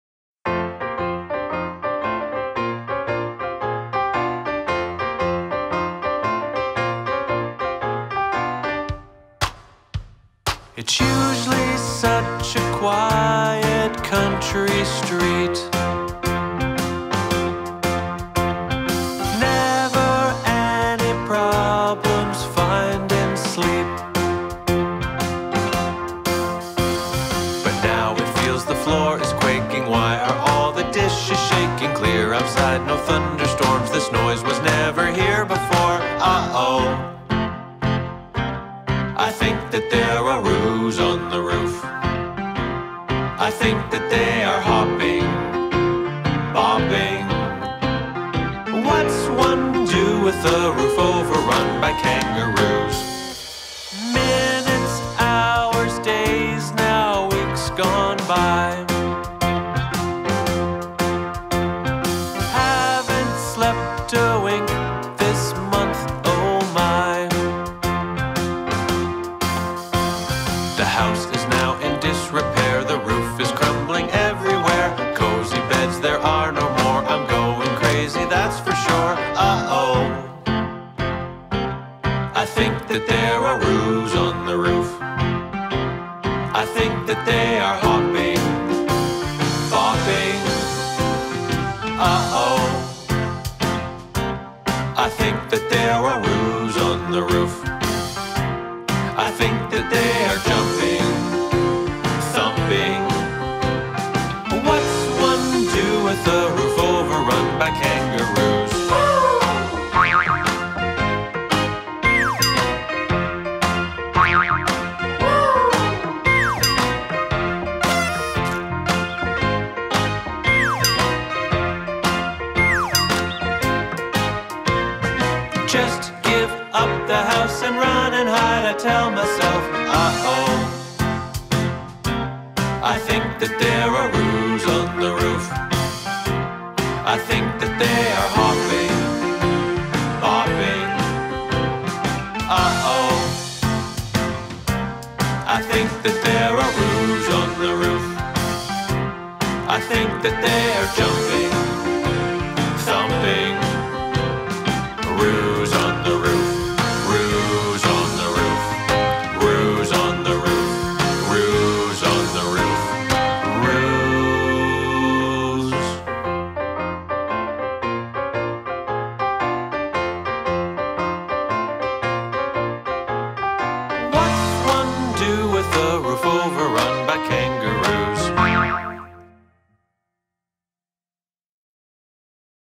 'Roos on the Roof! - Jonathan Monk
Just when you wanted to get a good night's sleep.  Alas!   What's one do with a roof overrun by kangaroos?